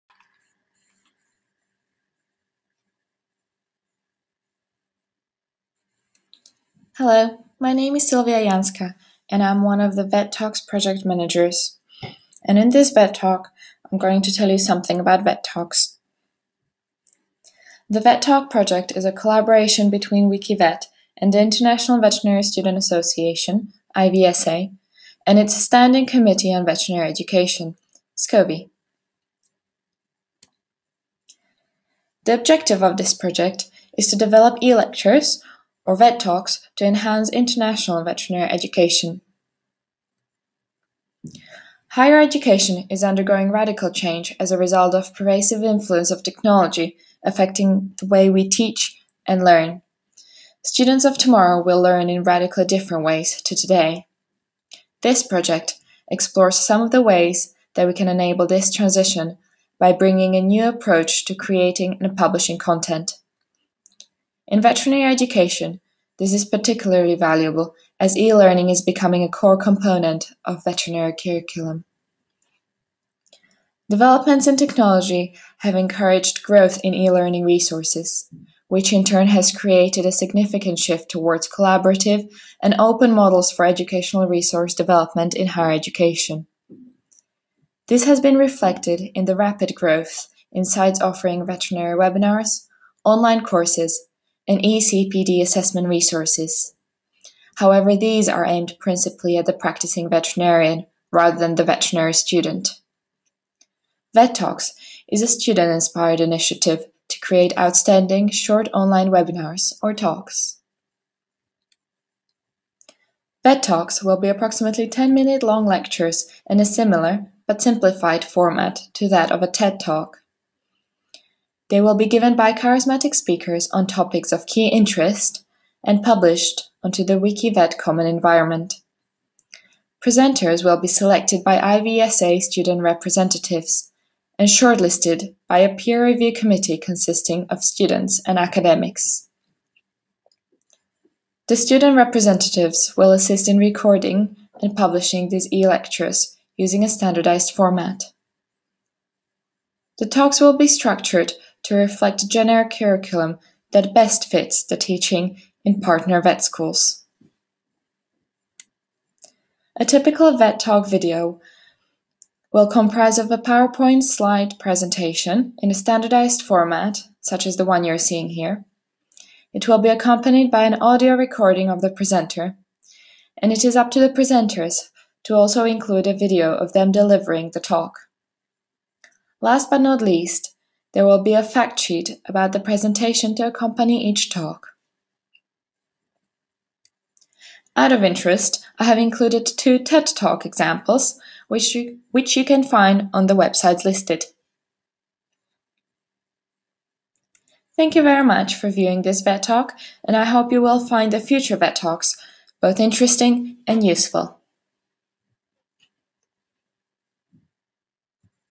About VET Talks Project
VET Talks is a project by the IVSA Standing Committee on Veterinary Education (SCoVE).

This VET Talk is by Silvia Janská, BSc(Hons), MSc, MRCVS, Project Manager, VET Talks Coordination Team 2015, RVC, University of London (UK)